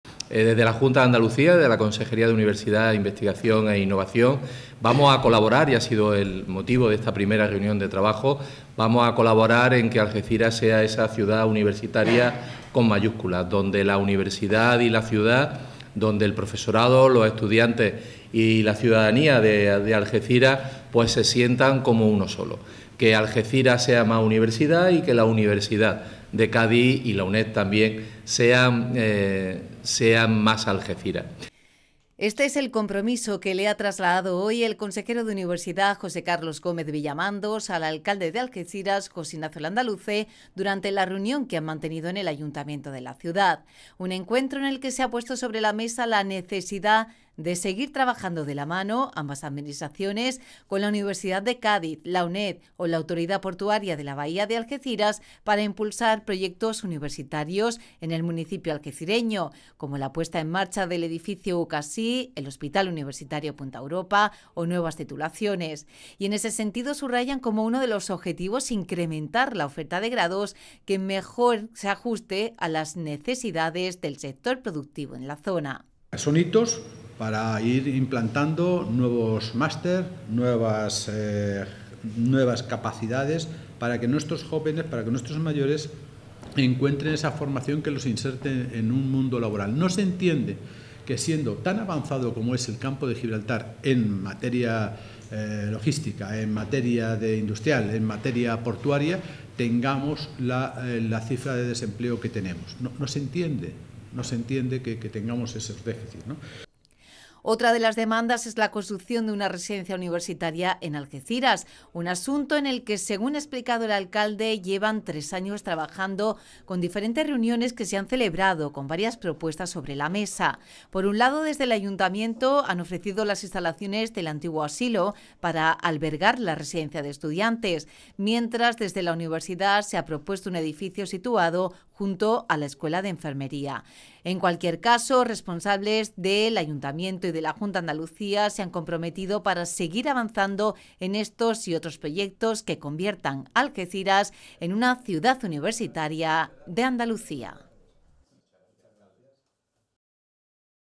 Junta y Ayuntamiento, comprometidos en convertir Algeciras en ciudad universitaria
'Convertir Algeciras en una ciudad universitaria con mayúsculas' este es el compromiso de la Consejería de Universidad, Investigación e Innovación de la Junta de Andalucía cuyo máximo responsable, José Carlos Gómez Villamandos, se ha reunido hoy con el alcalde, José Ignacio Landaluce, en el consistorio algecireño. Entre los temas que se han tratado en el encuentro, destaca la necesidad de implantar nuevos grados universitarios y la construcción de una residencia para estudiantes.